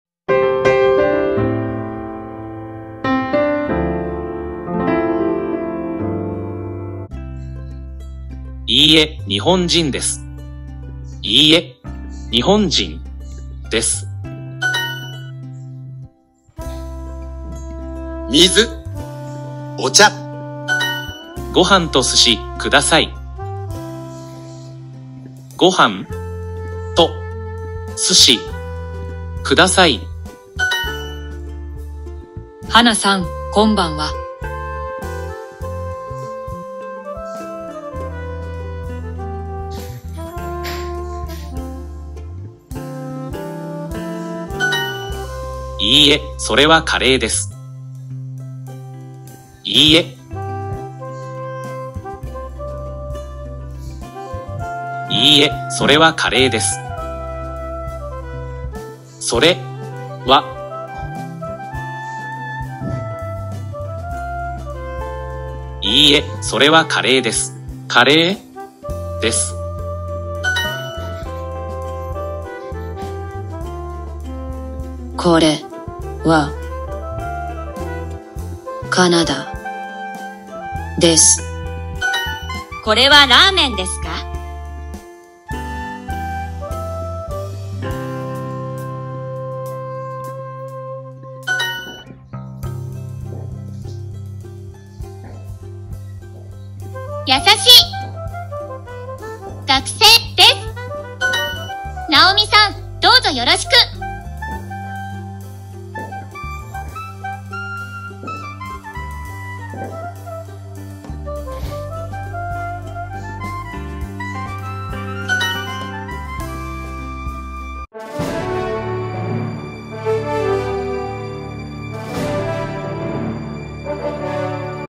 Learning Japanese Day 80: Introduction to food and greeting
Day 80 summary:

Gohan to sushi, kudasai: ご飯と寿司，ください； definition: Rice and sushi, please.
Hana san, konbanwa: はなさん，こんばんわ； definition: Good evening, Hana.
Iie, Nihonjin desu:　いいえ，日本人です； definition: No, she’s Japanese.
Iie, sore wa karee desu: いいえ，それは辛えです； definition: No, that’s curry.
Kore wa kanada desu: これはカナダです； definition: This is Canada.
Kore wa raamen desu ka: これはらアメンですか； definition: Is this ramen?
Mizu, ocha: 水，お茶； definition: Water, green tea.
Naomi san, dou zo yo ro shi ku: 直美さん；どうぞ宜しく； definition: Nice to meet you, Naomi.
Yasashii gakusei desu: 優しい学生です； definition: A nice student.